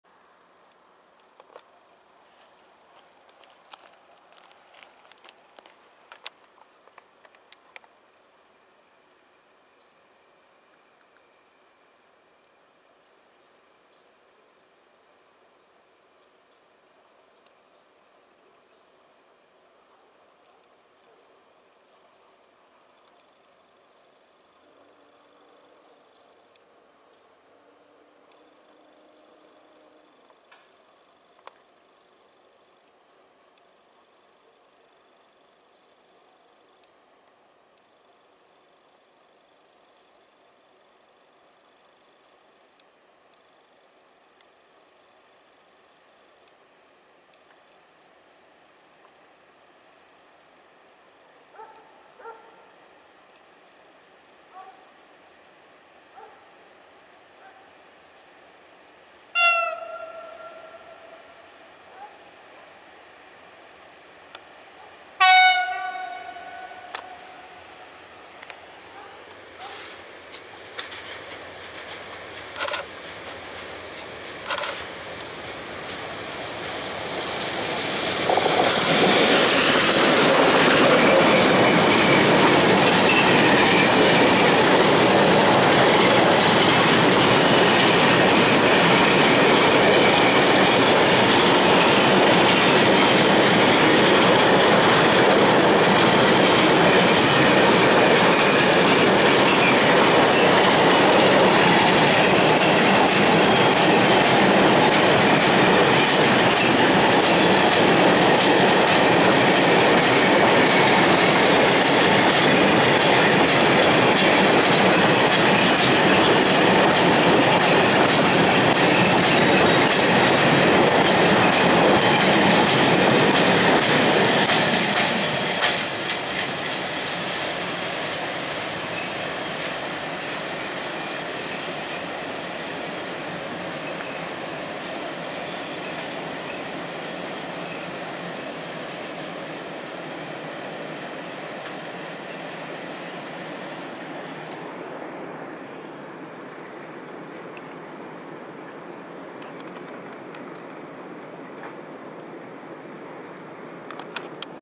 Byczek o zachodzie słońca ;)
Przejazd przez Grabce Wręckie ET22-1026 z krytymi w stronę Puszczy Mariańskiej. Podziękowania i pozdrowienia dla mechanika za RP1 :). Przy okazji załapał się na piękny zachód słońca :). Data nagrania: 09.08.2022